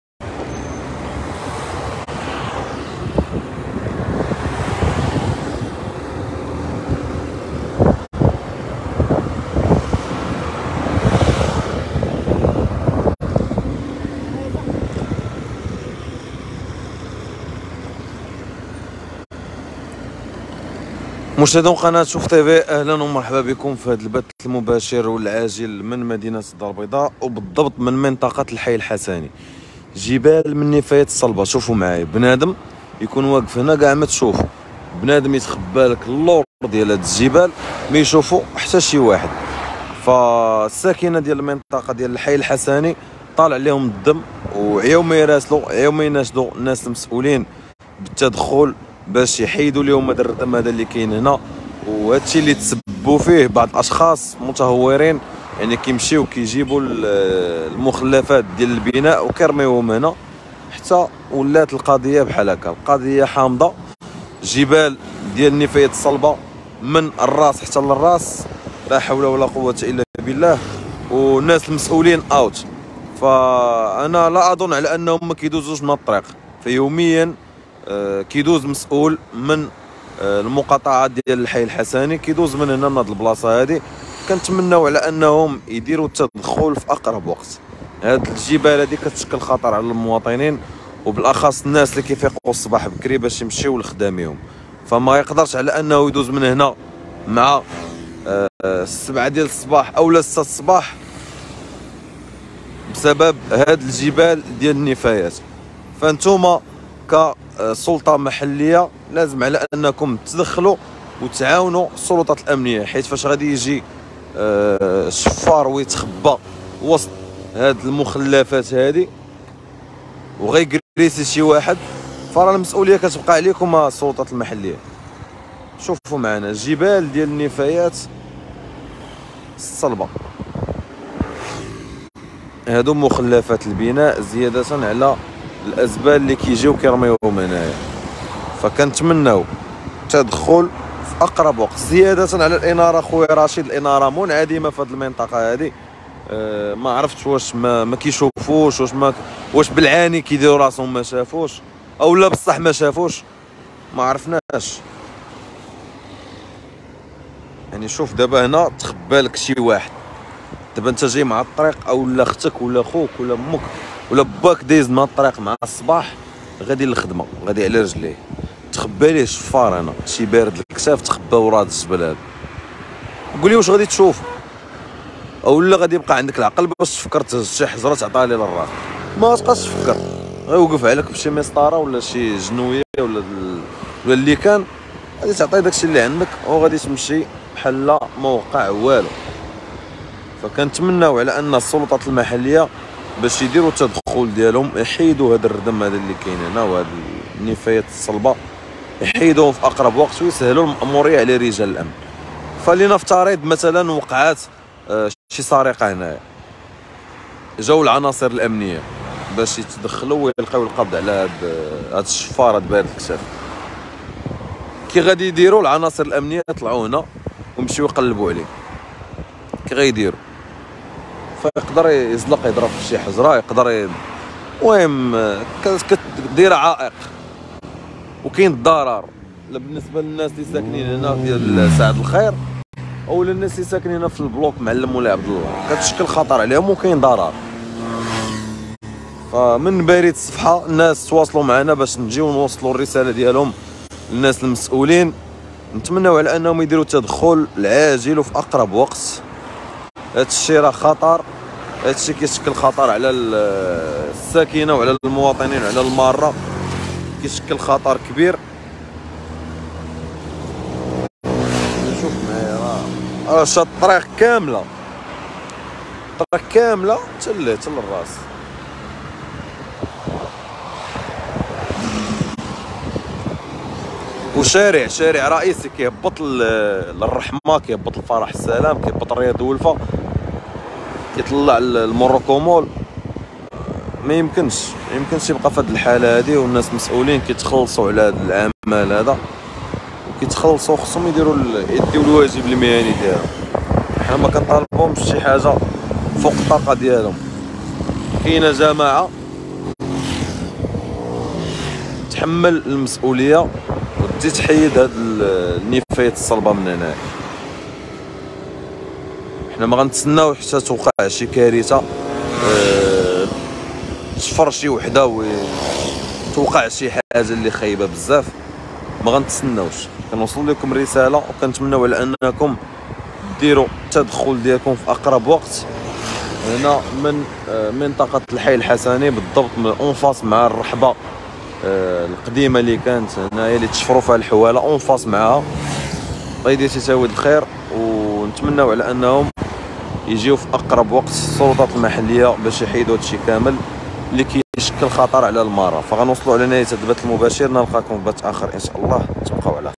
من البيضاء..جبال من النفايات الصلبة تثير غضب ساكنة الحي الحسني والمسؤولون آوت..شوفو المشهد الصادم
مباشرة من الدارالبيضاء..جبال من النفايات الصلبة تثير غضب ساكنة الحي الحسني والمسؤولون آوت..شوفو المشهد الصادم